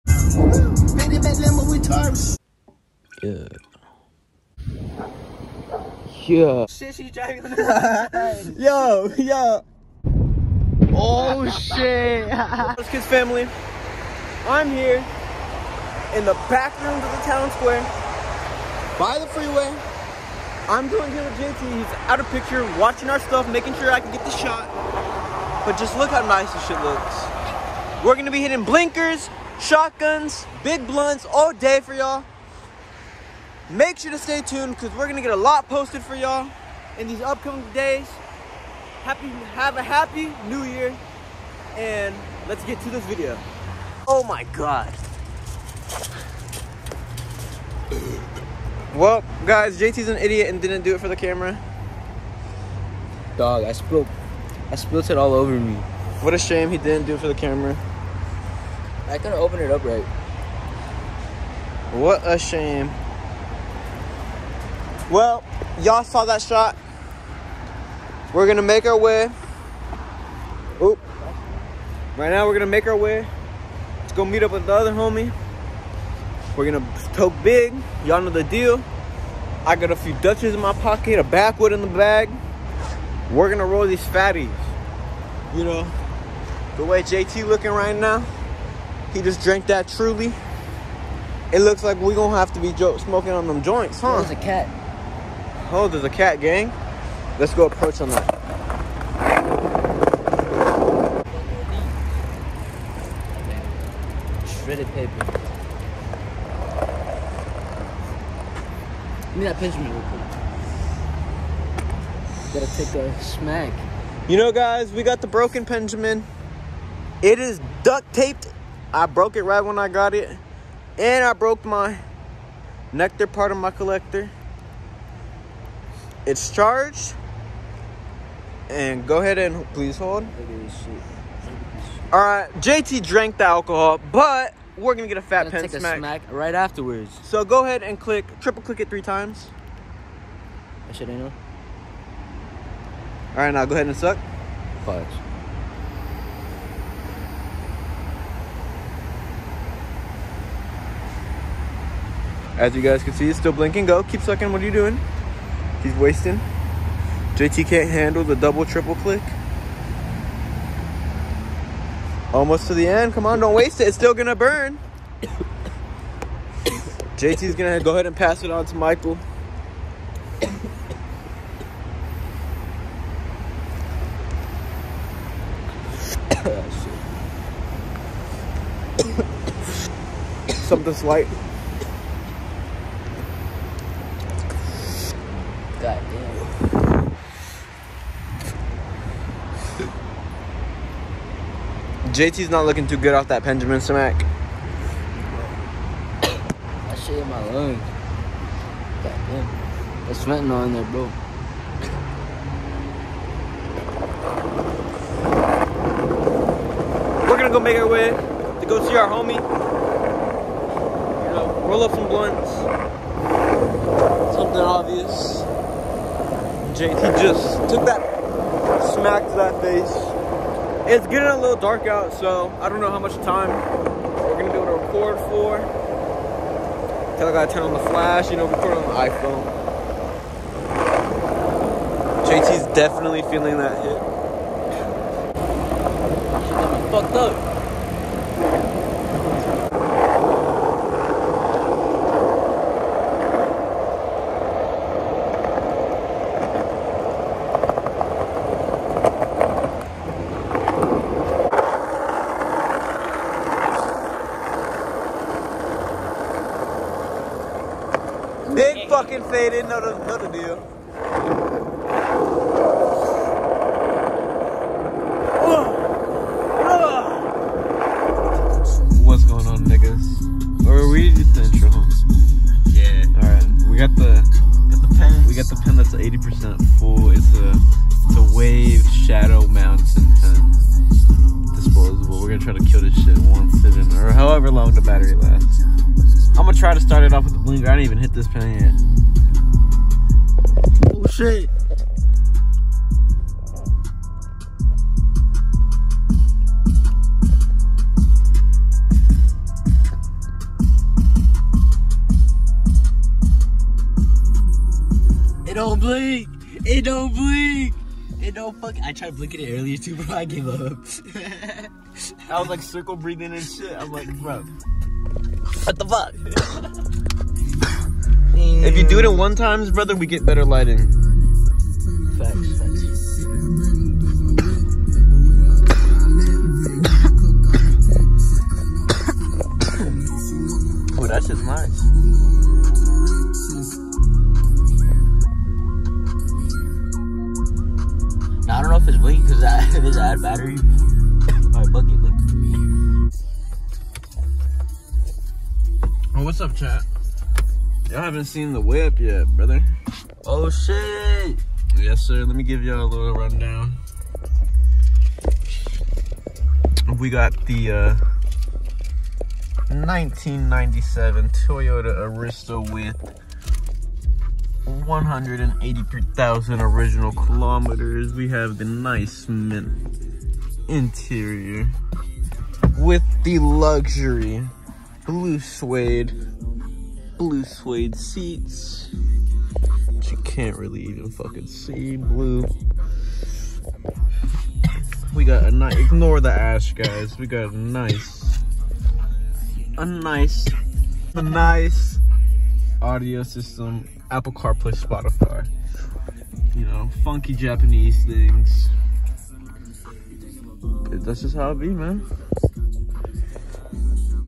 HAPPY NEW YEAR 2024 (scrapped videos)
Edited this off shrooms, this video is just scrapped recordings I found that I never got a chance to work on until now    -JT

18+

smoke, pens, weed, dabs, 420, 710